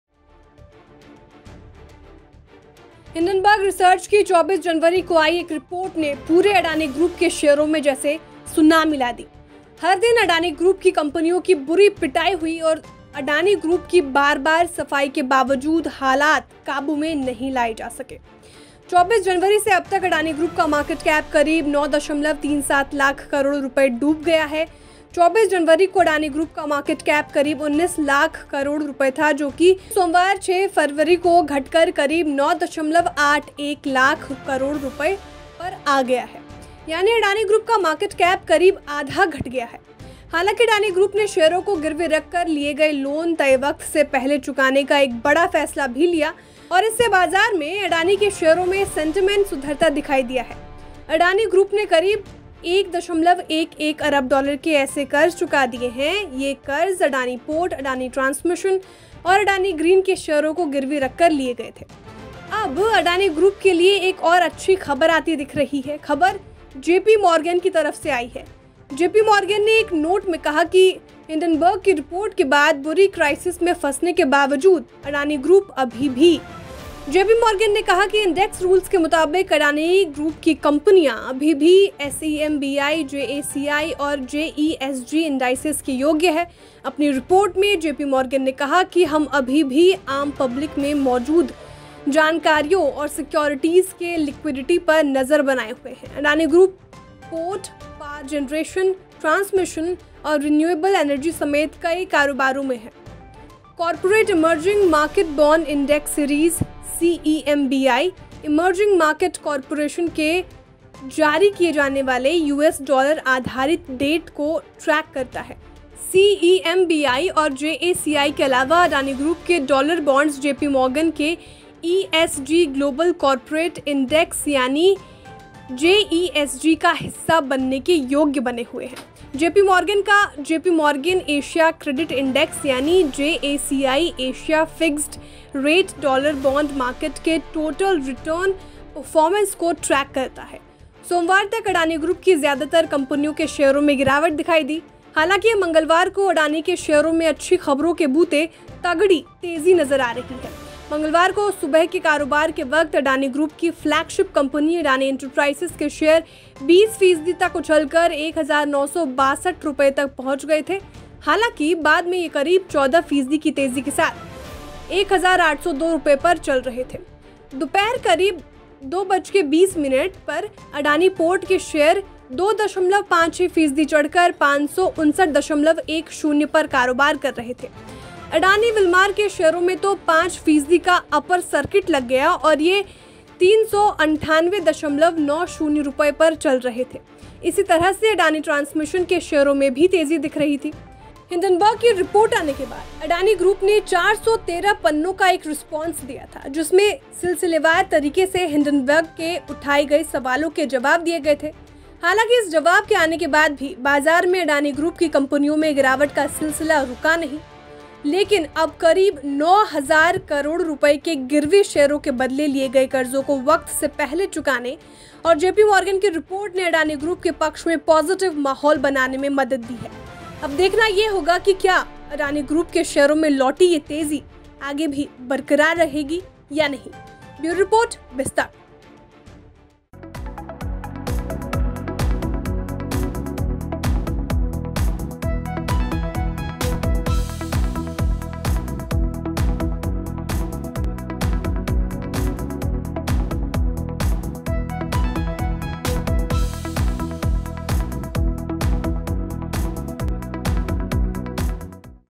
Adani Group पर JP Morgan की सबसे बड़ी रिपोर्ट ! |BIZ Tak
Adani Group still eligible for CEMBI, JACI, JESG indices: JP Morgan
Watch this video for full details.